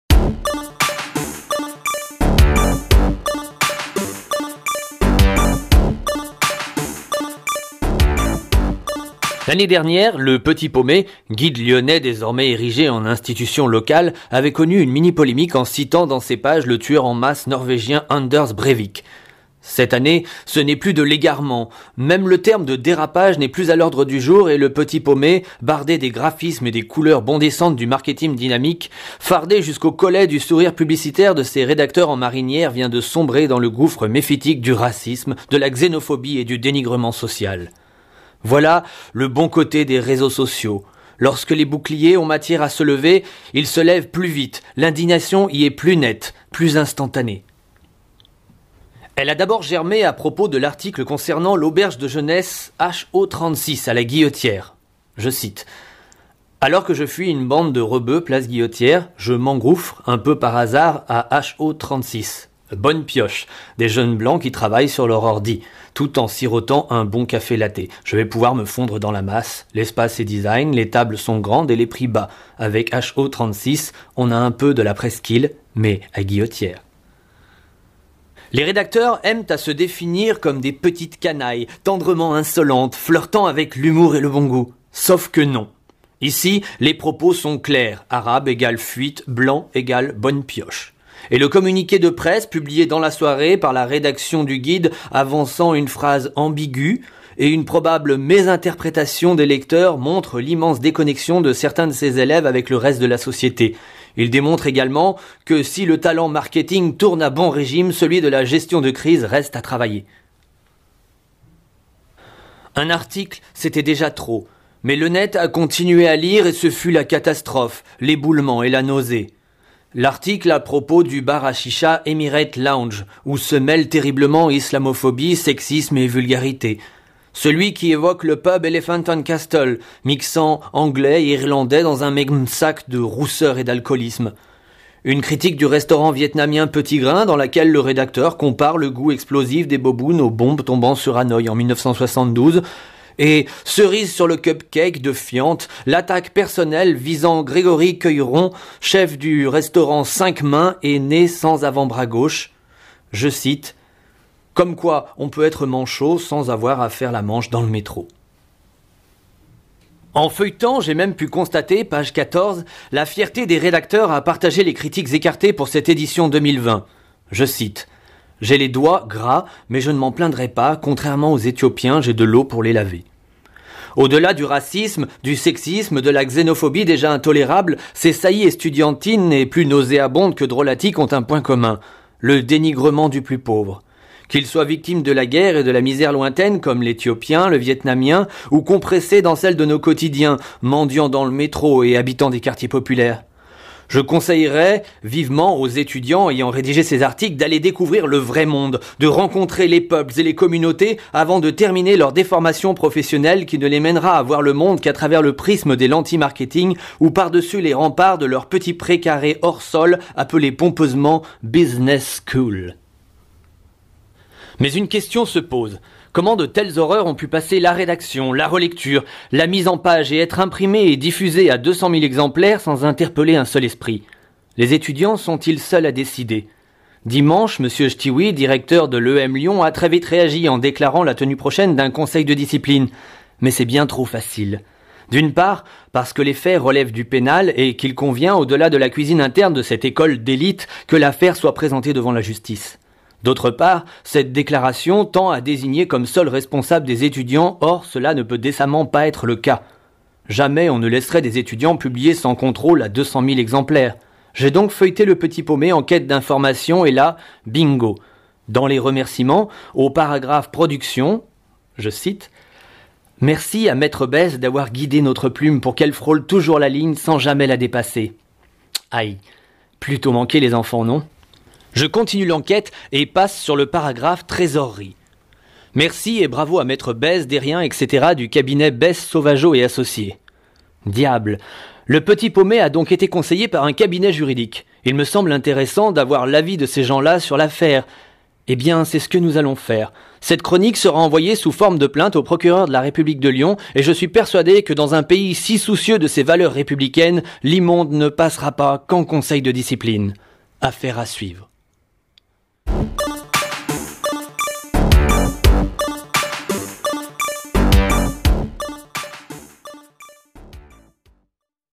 Petit Paumé, grande nausée
Polémique autour de la nouvelle édition du guide lyonnais "Le Petit Paumé"
N'hésitez pas à réagir en commentaire.